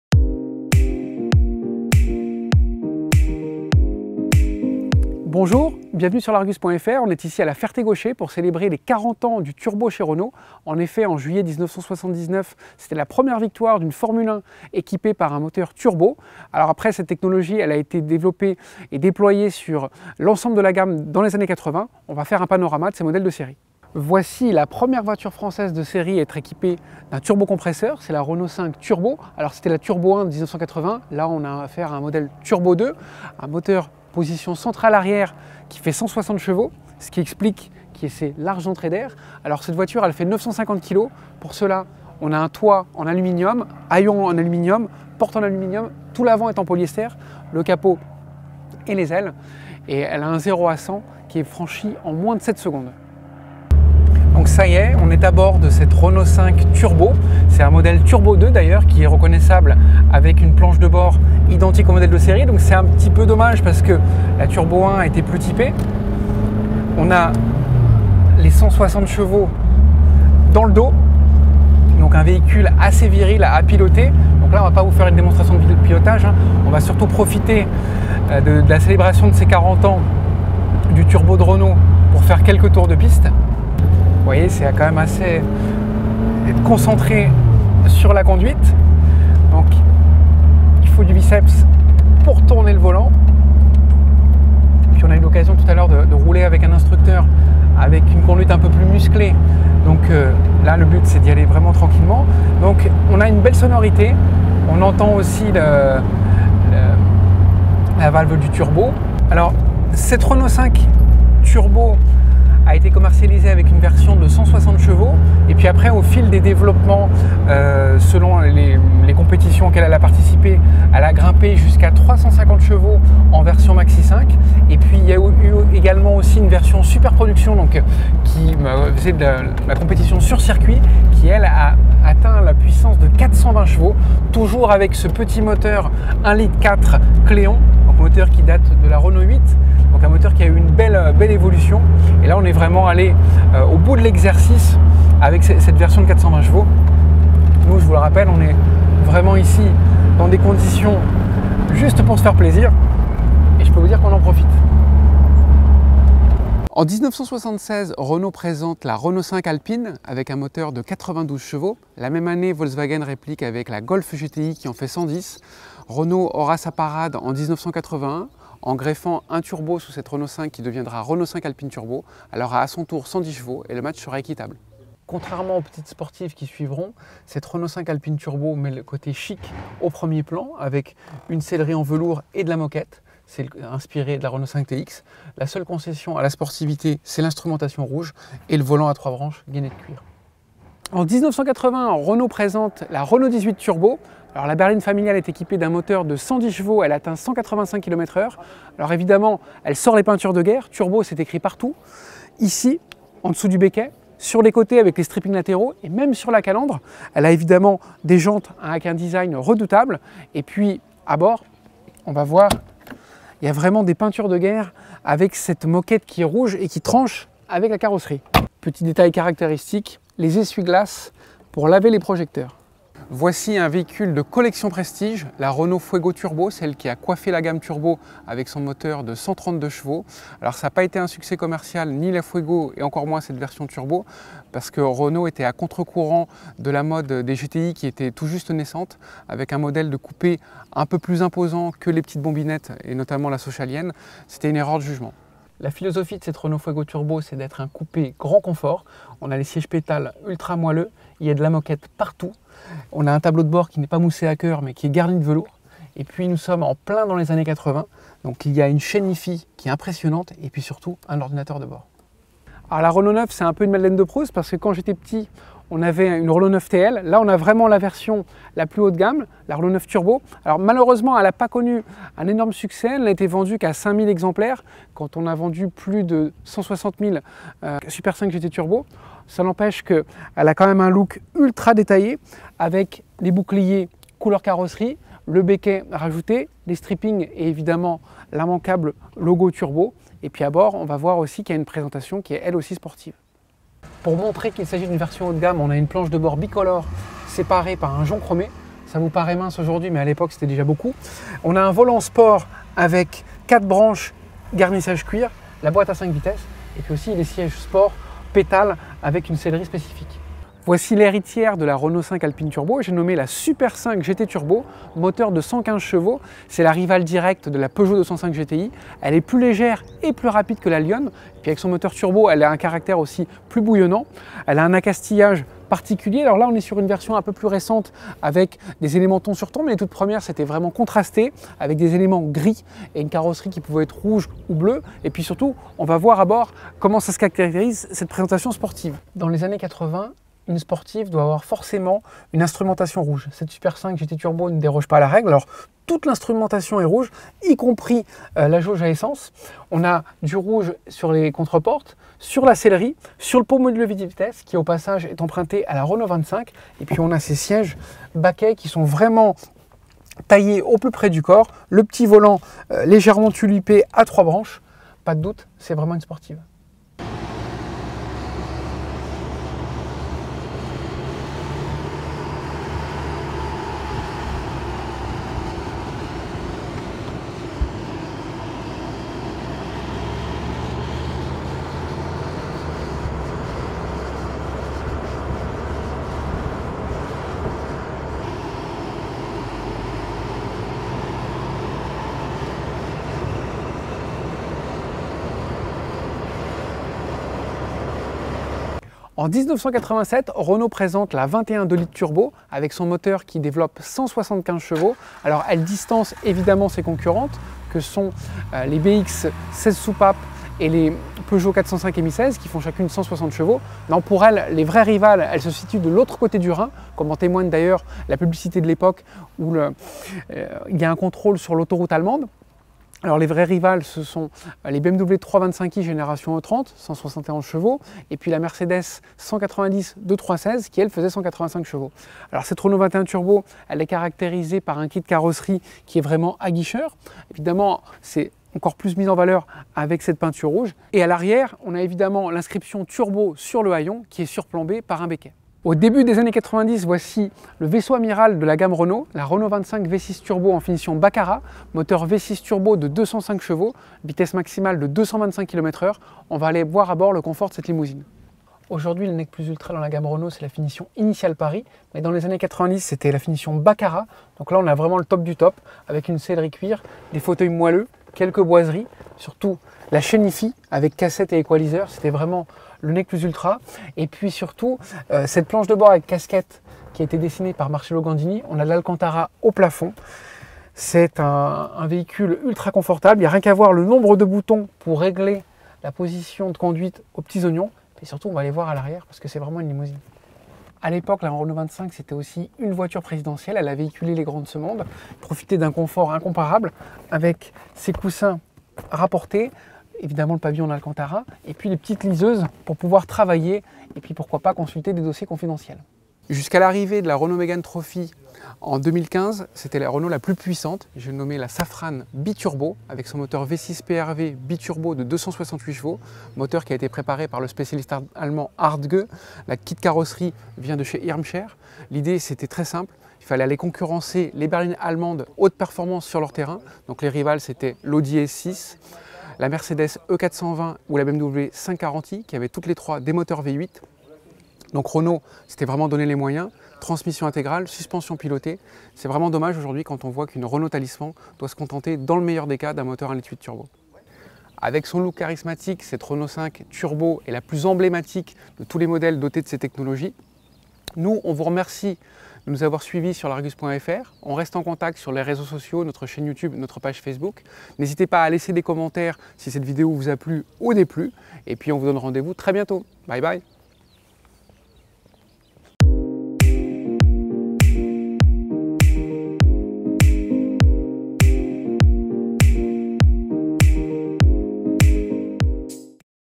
Rédécouvrez les Renault à moteur Turbo des années 1980 et 1990
De la Renault 5 Turbo, à la Safrane Biturbo en passant par les R9, R11, R21 et R25, sans oublier la Fuego, retour sur des véhicules aujourd'hui collector.